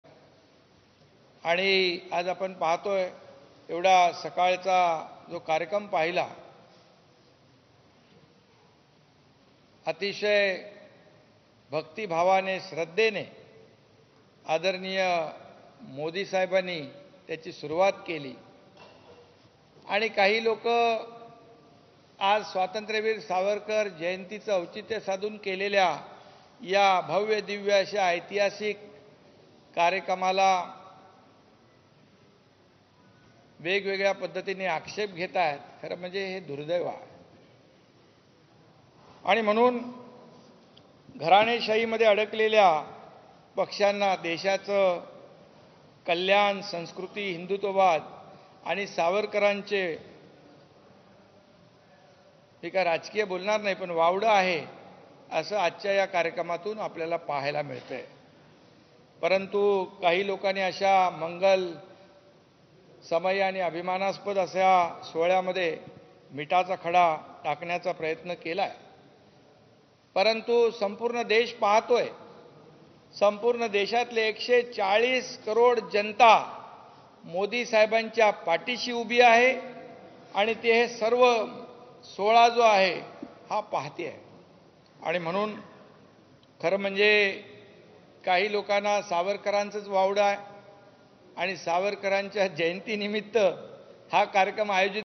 Eknath Shinde : नव्या संसदभवनाच्या इमारतीच्या कार्यक्रमावर आक्षेप, हे दुर्दैव आह- मुख्यमंत्री
LIVE: PM Modi Inaugurates New Parliament Building | Central Vista Project